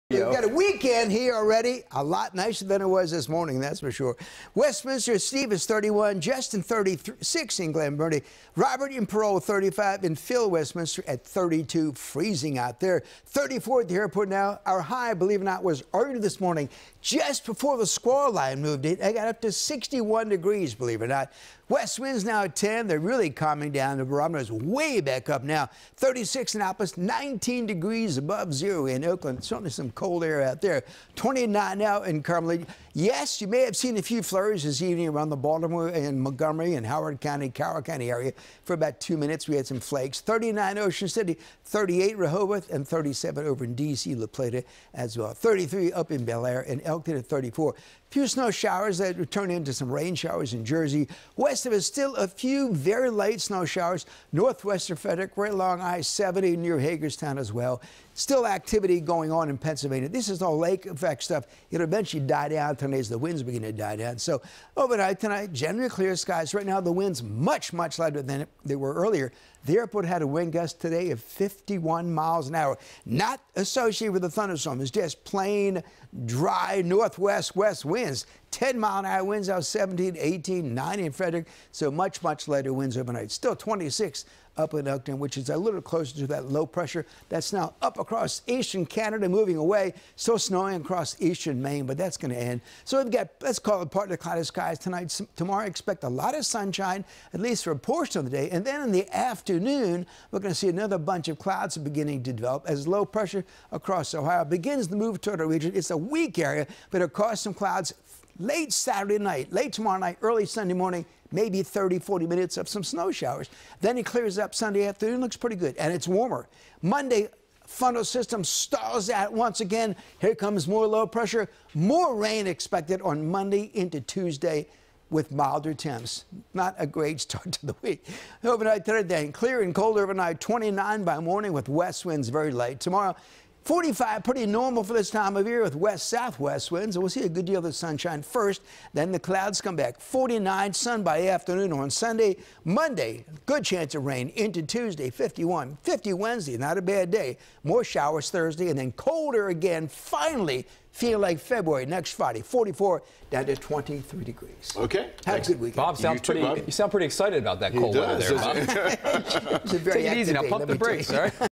Bob Turk Has One Last Look At Your Friday Night Forecast
It will be a return to normal for the weekend.